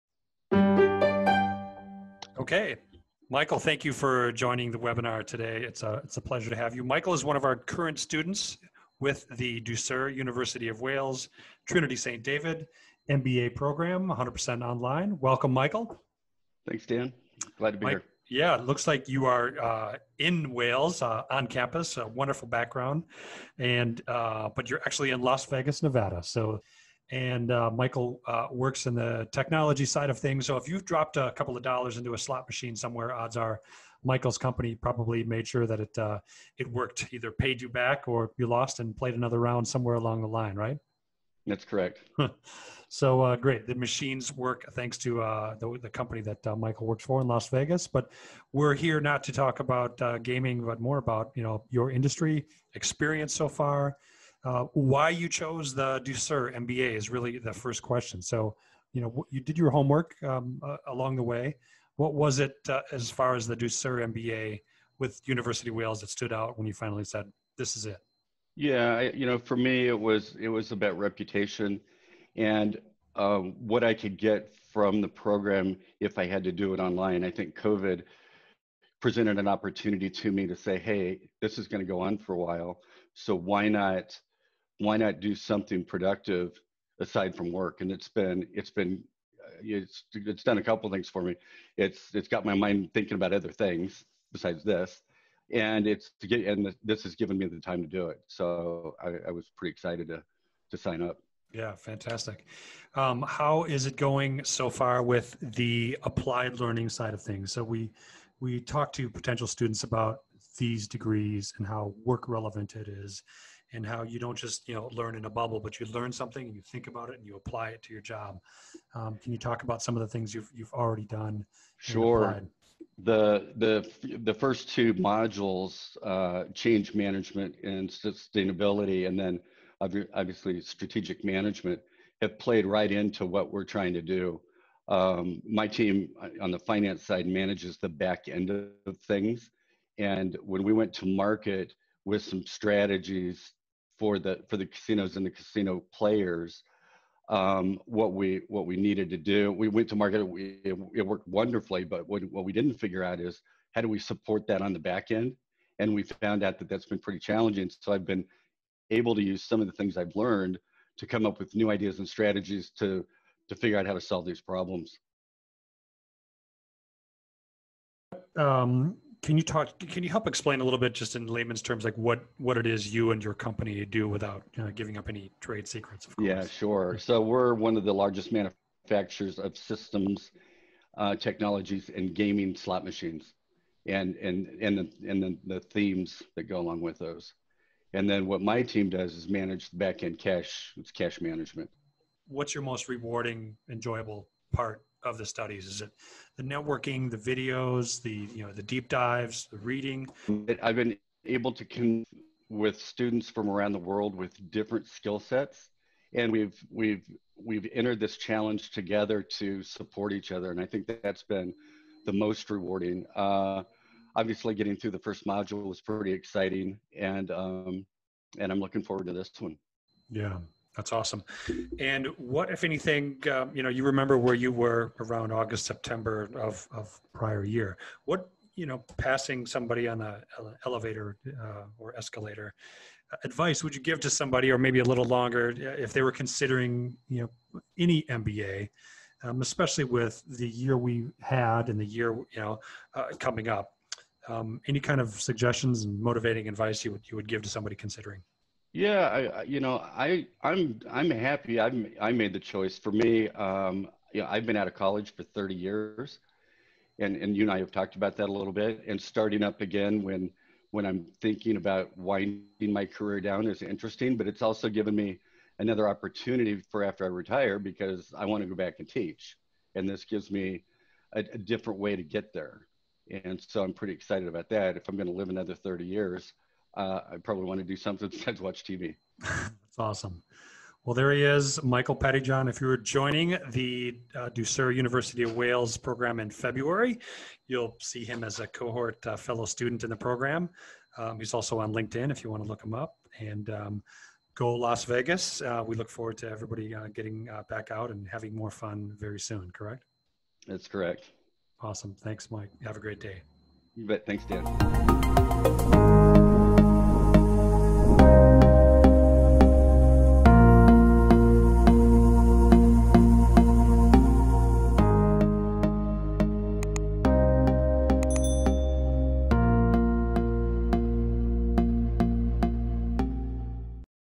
Jackpot! Getting the most out of your MBA while working in the gaming industry.
Michael Pettyjohn works for one of the world's largest gaming companies and is a current Ducere MBA student. Michael shares why he chose Ducere and how he is applying current studies to your job.